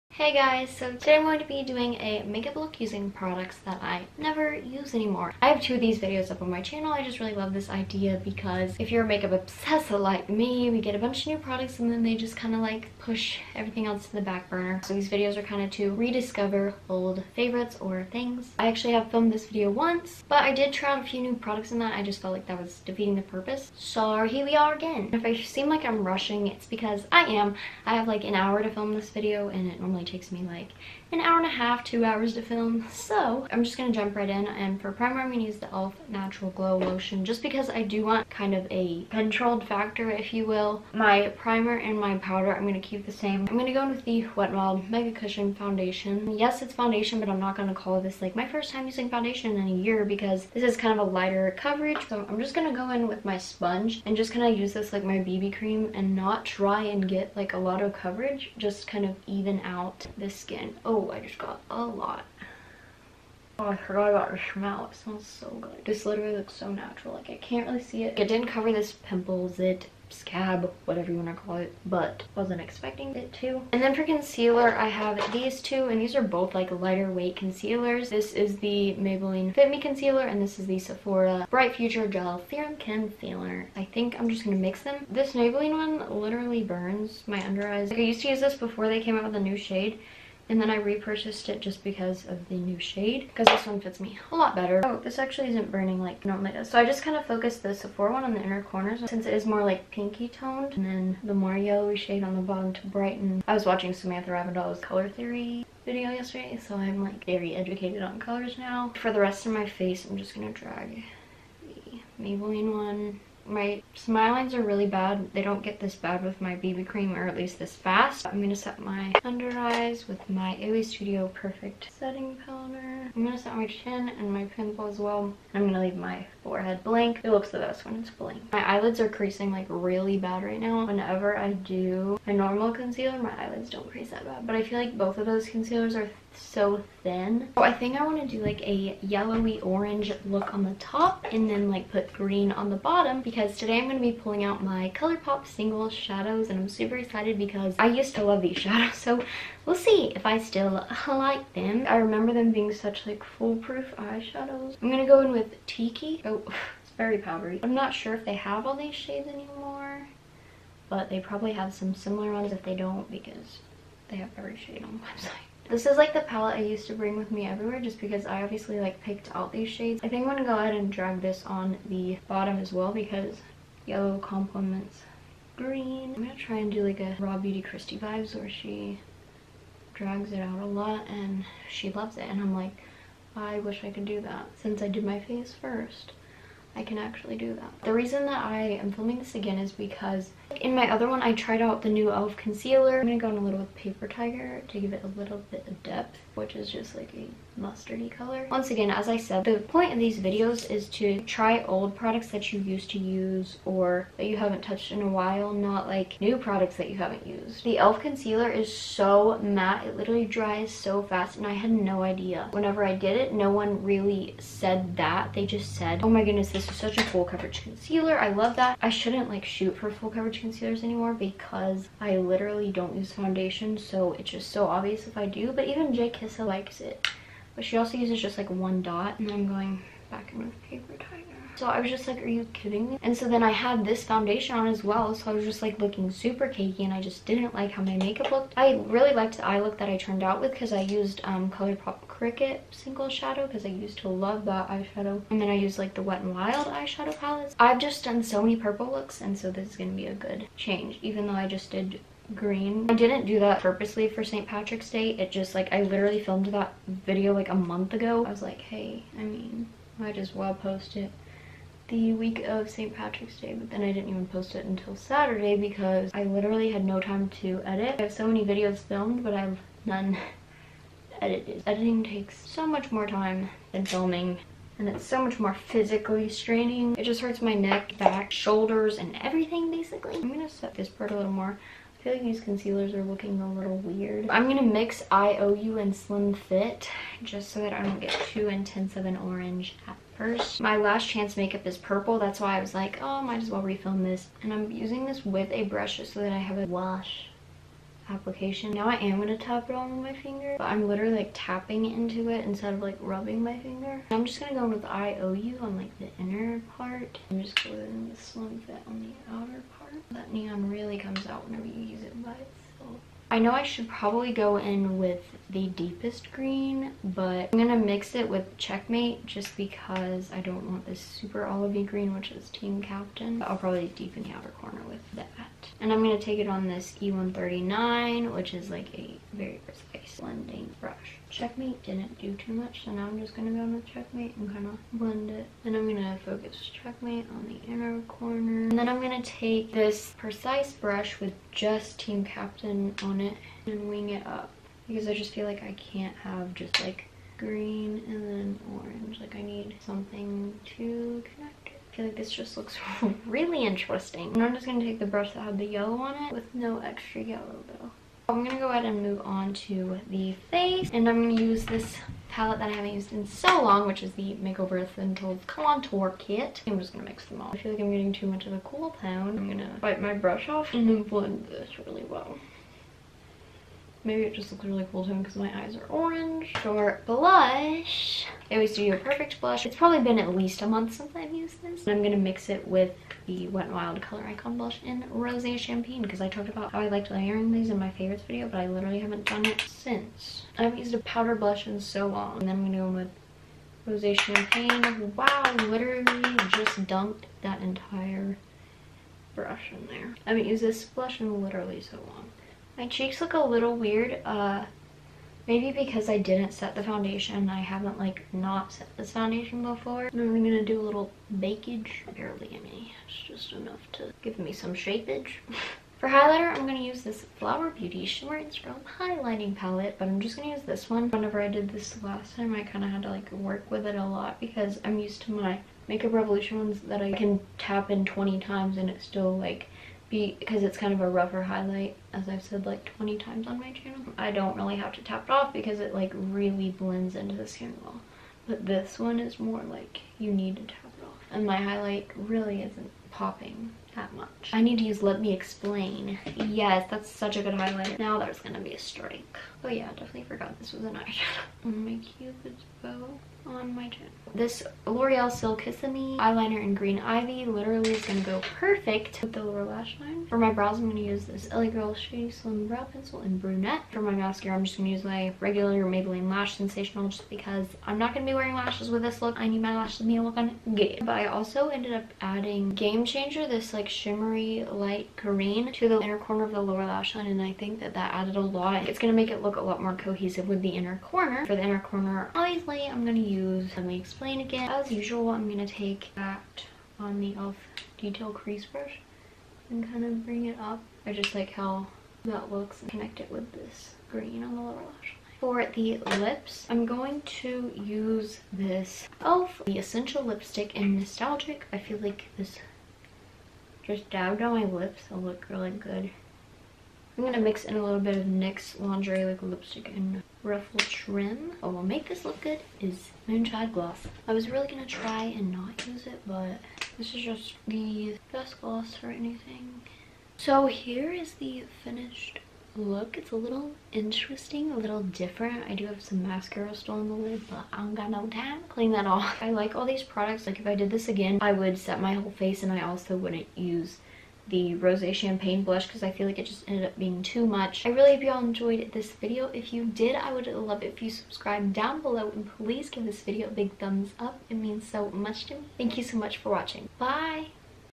FULL FACE USING PRODUCTS I NEVER USE!
I always forget about my old products.. so here is a fun video to remember them!

Products Mentioned:

Elf Natural Glow Lotion
Wet n Wild Mega Cushion Foundation
Maybelline Fit Me Concealer
Sephora Bright Future Gel Serum Concealer
AOA Studio Perfect Setting Powder
Colourpop Pressed Powder Shadows - Tiki, Paper Tiger, I Owe You, Slim Fit, Checkmate, Team Captain, Let Me Explain, Game Changer
Makeover Essentials Contour Kit
AOA Studio Perfect Blush -
Wet n Wild Color Icon Blush - Rose Champagne
flower beauyt Shimmer and Strobe Highlighter Palette
L'Oreal Silkissme Eyeliner - Green Ivy
LA Girl Shady Slim Brow Pencil
Maybelline Lash Sensational Mascara
Elf Essential Lipstick - Nostalgic
Nyx Lingerie Liquid Lipstick - Ruffle Trim
Colourpop X Kathleenlights Moonchild Gloss

What I'm Wearing:

Tank - Target
Necklace - Kendra Scott